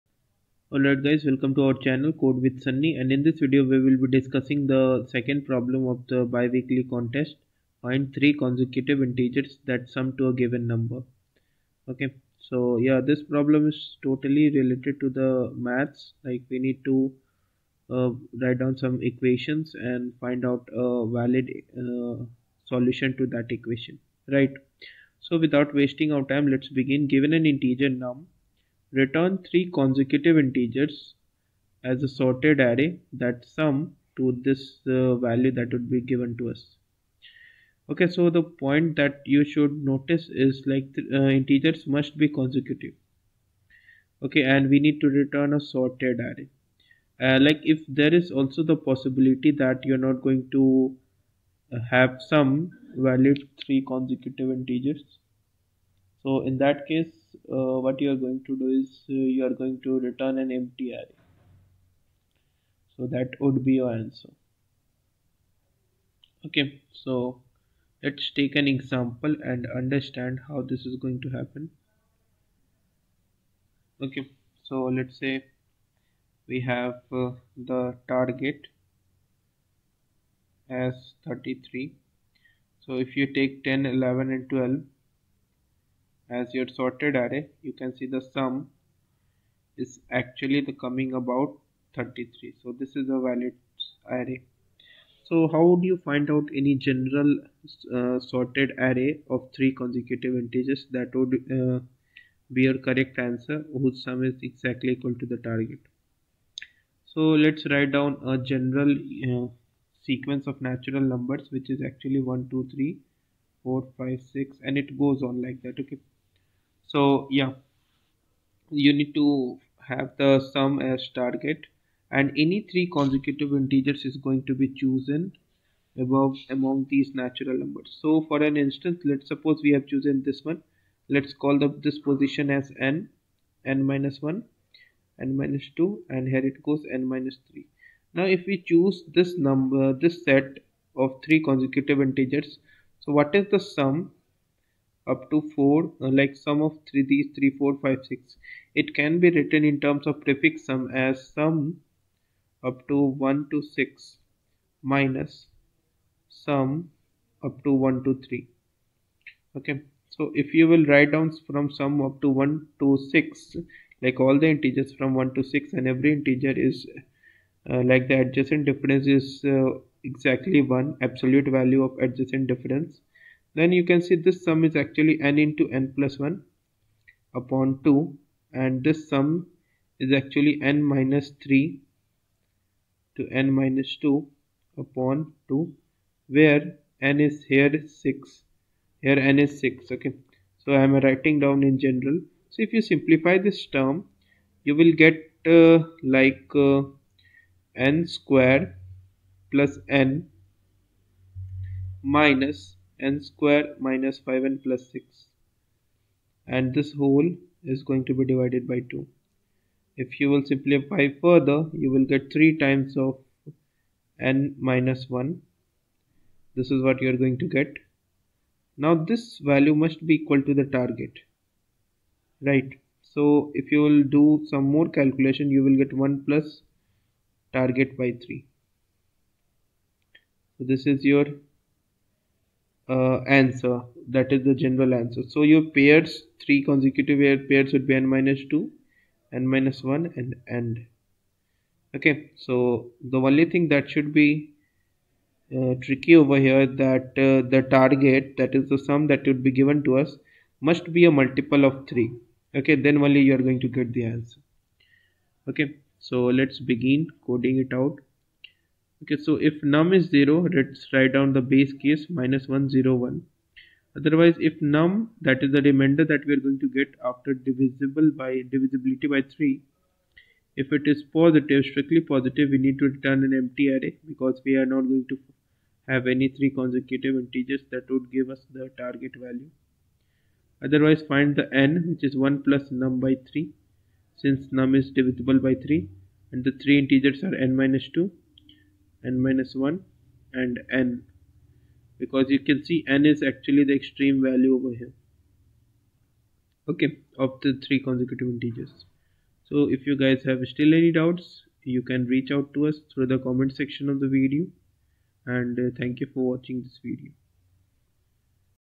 Find Three Consecutive Integers That Sum to a Given Number | 2177 LeetCode | BiWeekly Contest 72
Here is the detailed solution of the LEETCODE FIND THREE CONSECUTIVE INTEGERS THAT SUM TO A GIVEN NUMBER Problem of the Leetcode BiWeekly Contest 72 and if you have any doubts, do comment below to let us know and help you.

In this video, I have talked about how to use some mathematics to solve the problem efficiently on O(1) time.

Fill out the below form, if you want one-to-one guidance/teaching:-